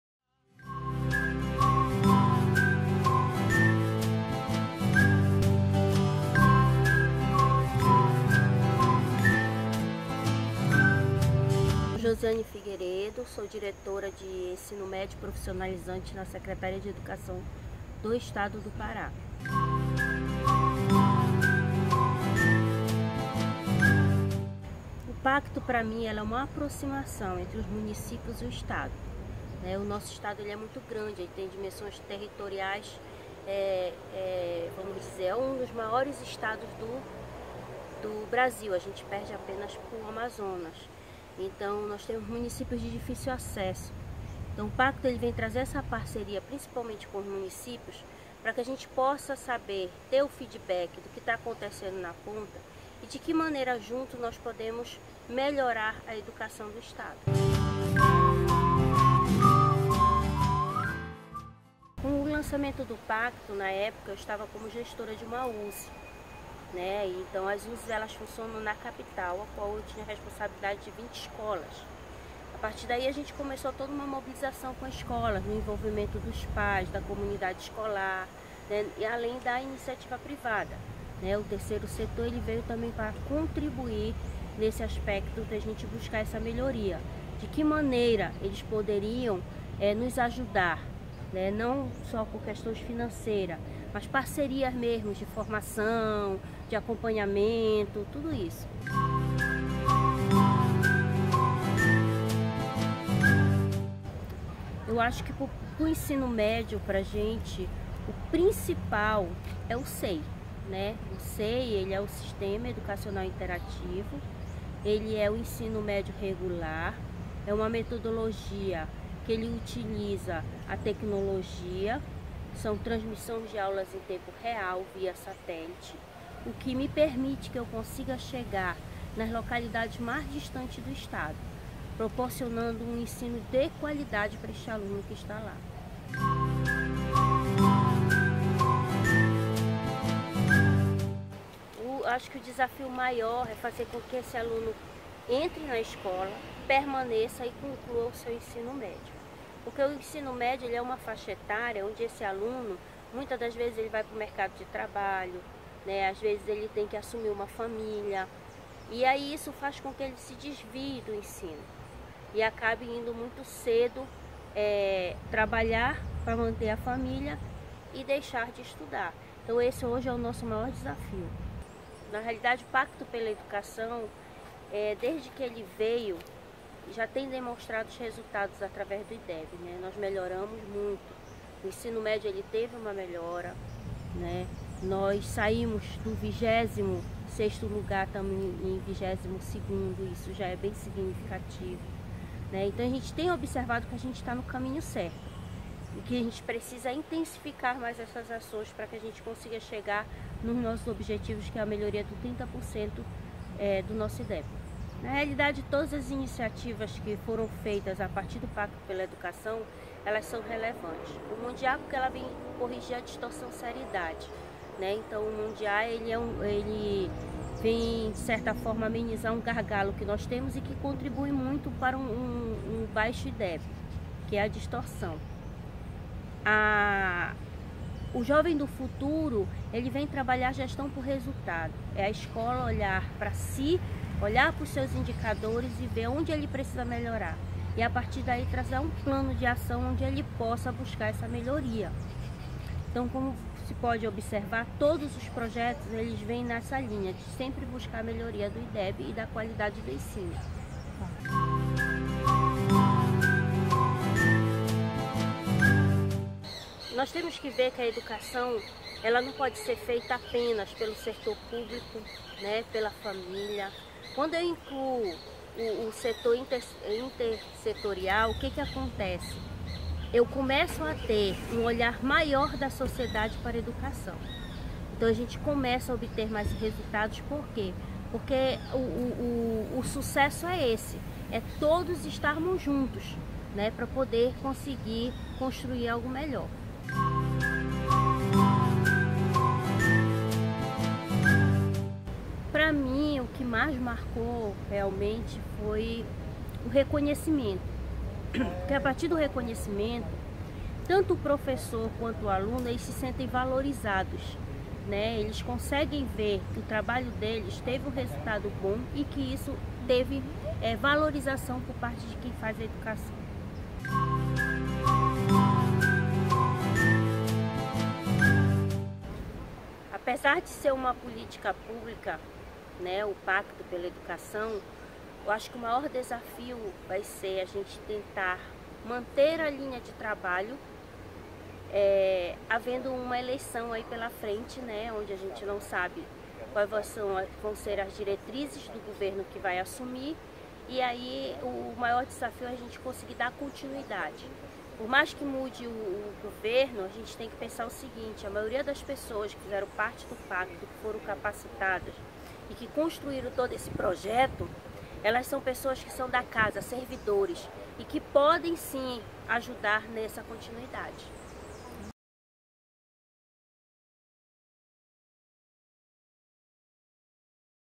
Joseane Figueiredo on the Pacto pela Educação do Pará in Brazil
Joseane Figueiredo, Director of High School and Professional Education in the Secretariat of Education of the State of Pará in Brazil, talks about the the Pacto pela Educação do Pará - a multi-stakeholder partnership to improve education in the state.